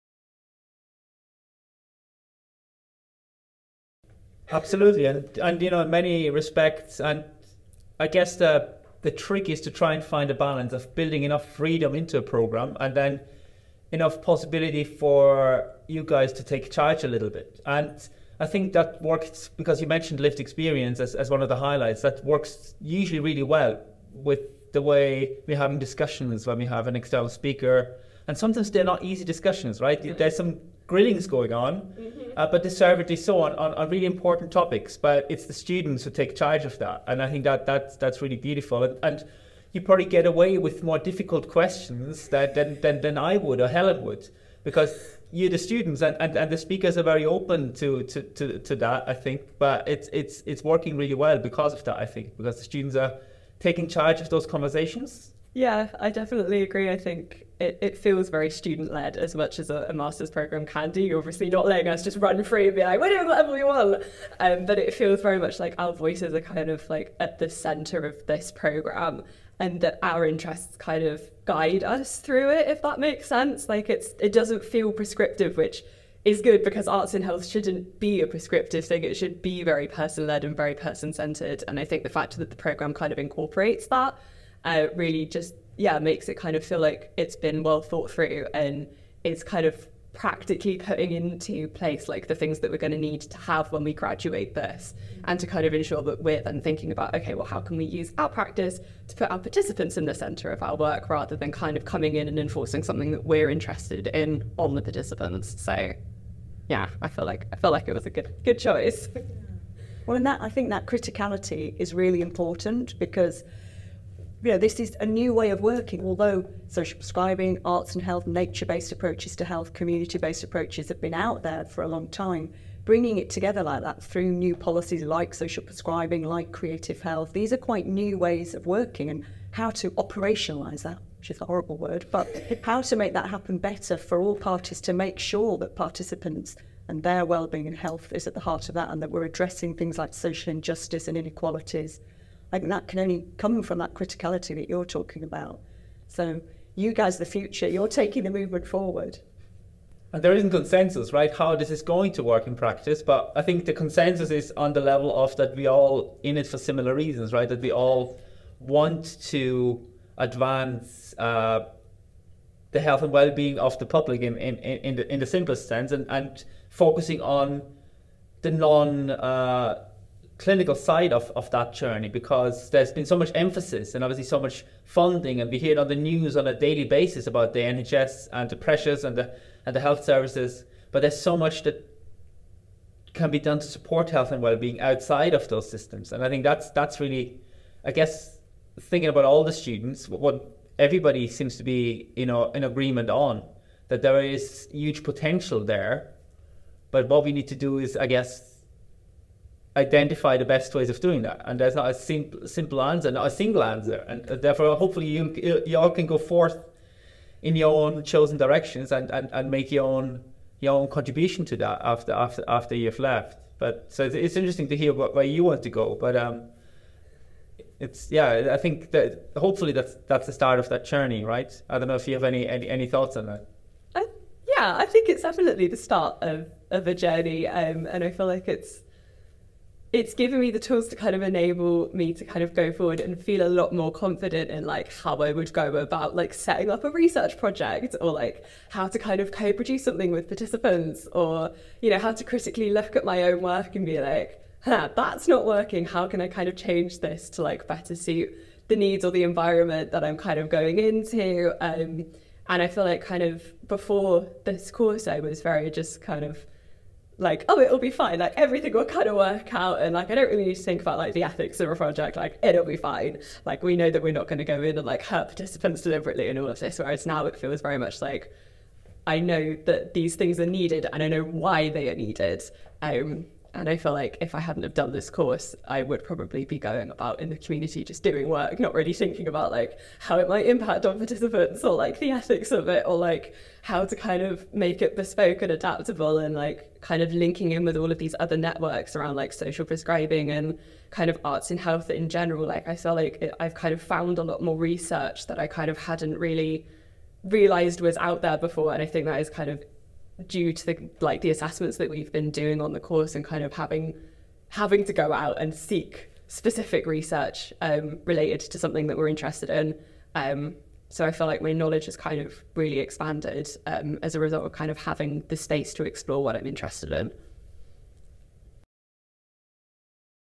Studying MASc Creative Health at UCL (Part 4)
In the final part of our series, former MASc student Beth talks to Prof Helen Chatterjee and Dr Thomas Kador about why she chose to study the MASc in Creative Health.

Find out more about the MASc in Creative Health, the first programme of its kind in the world, both in terms of the qualification (Masters in Arts & Sciences) and the academic field of study (Creative Health),